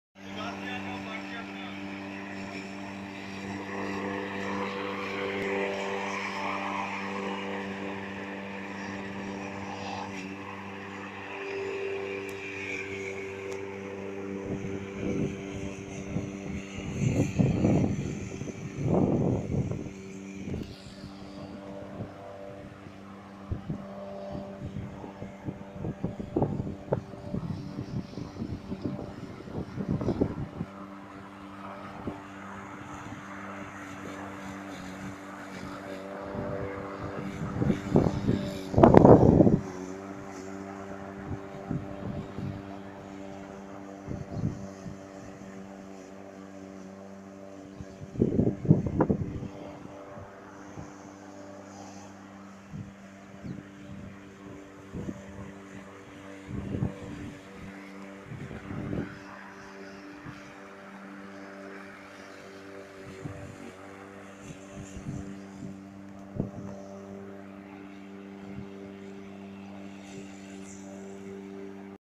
LawnKing electric lawn mower
Heavy Duty Commercial model with 18-inch cutting blade 1800 watt self protected motor. Steel body . 9 stage cutting height adjustable